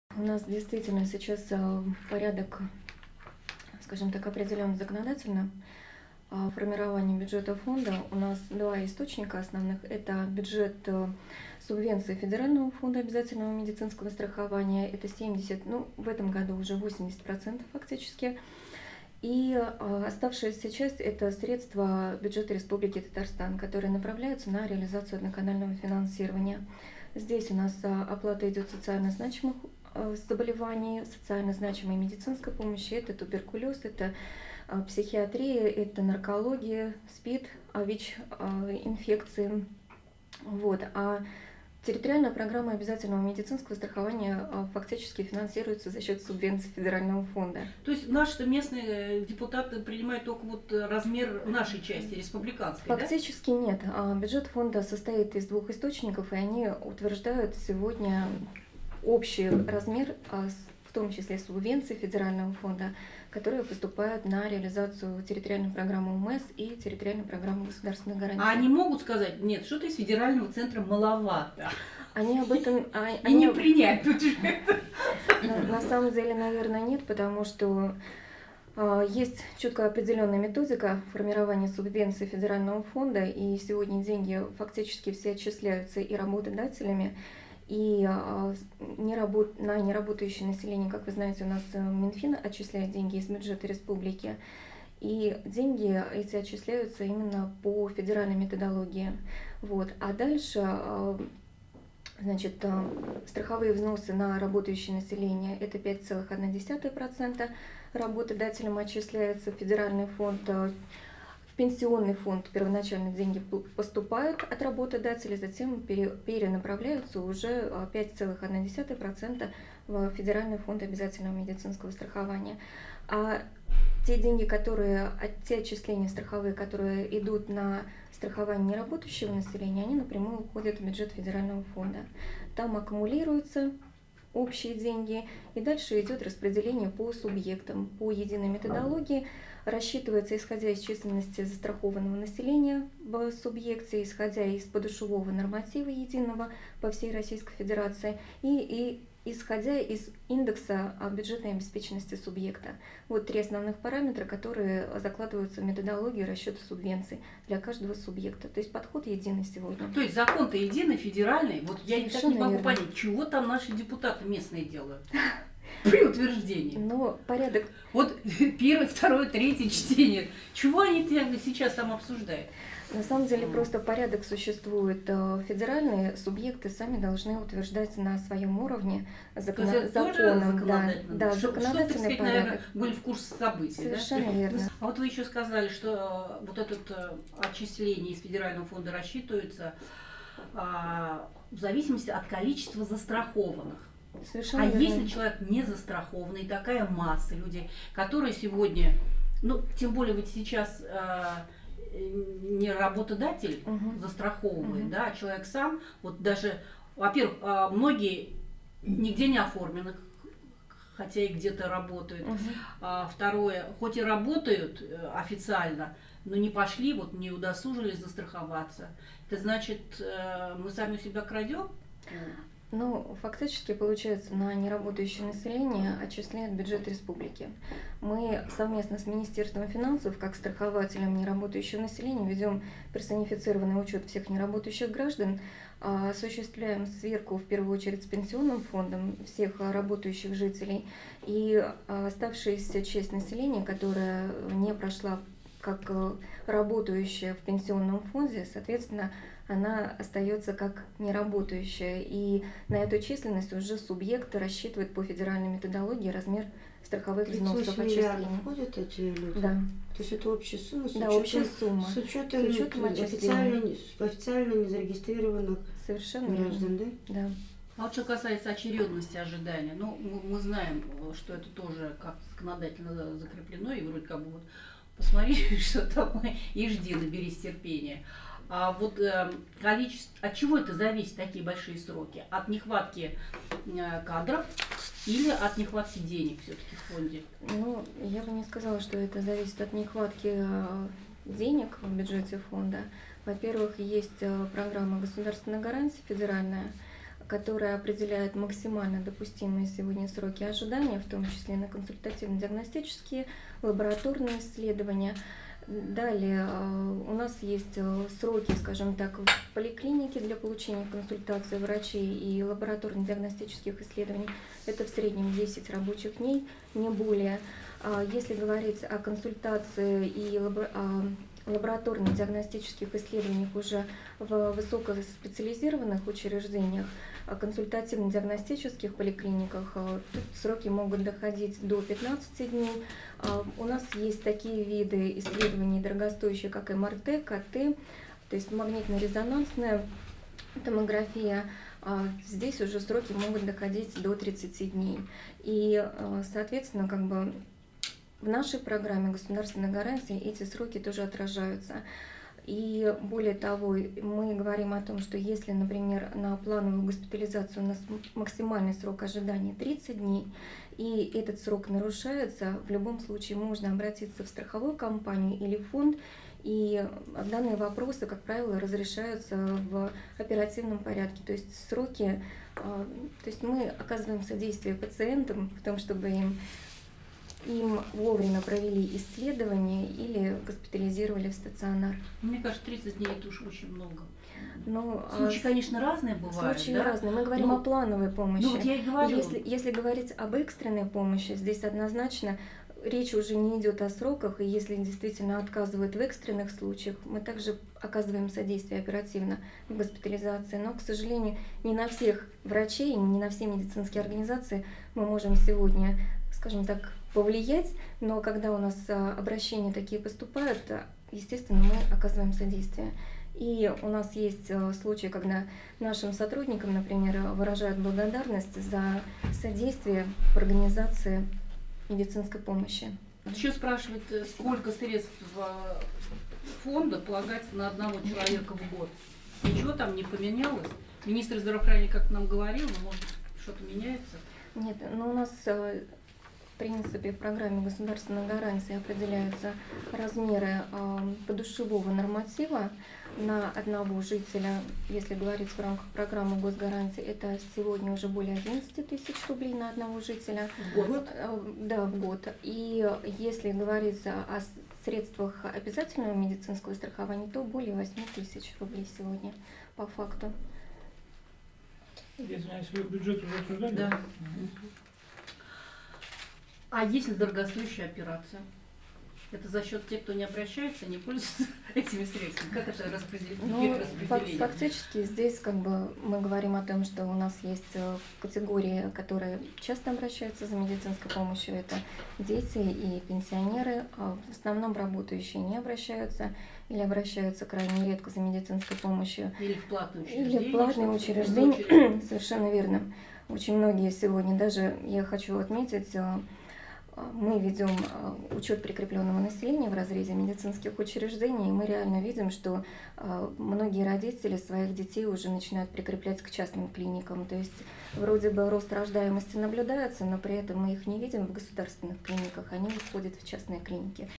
Алсу Мифтахова: Если говорить об экстренной помощи, то речь не идет о сроках
В 2015 году бюджет фонда обязательного медицинского страхования в Татарстане ожидается дефицитным — в связи с тем, что федеральные власти ставят перед российской медициной задачу делать упор на высокотехнологичное лечение.

Съемка и монтаж: Максим Тимофеев